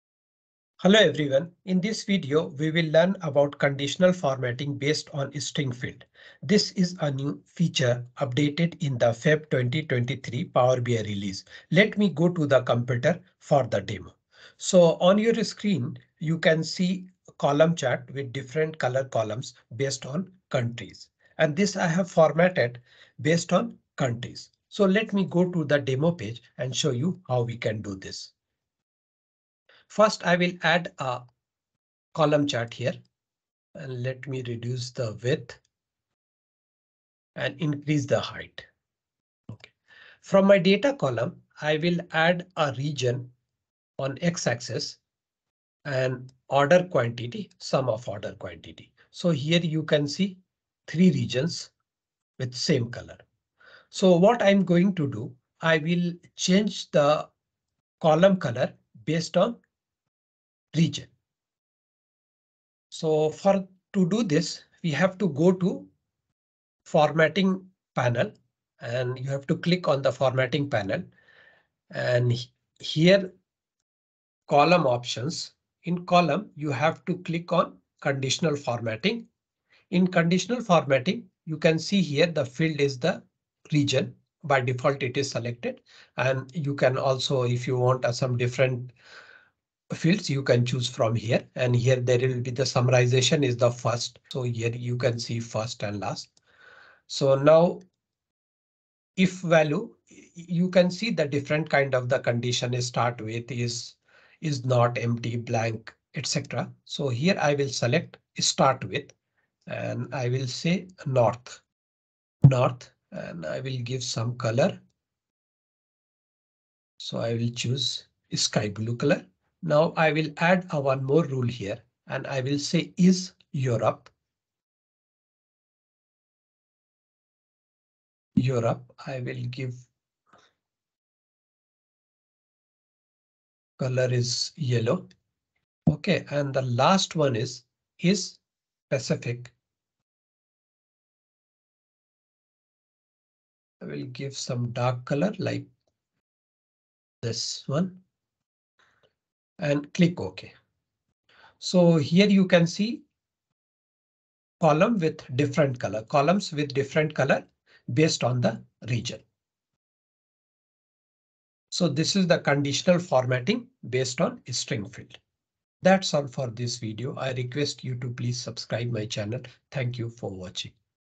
Conditional formatting based on string fields in Power BI - New feature
This is a new feature in Power BI that allows for conditional formatting based on string fields. Users can now apply formatting rules to their data visuals based on specific text values.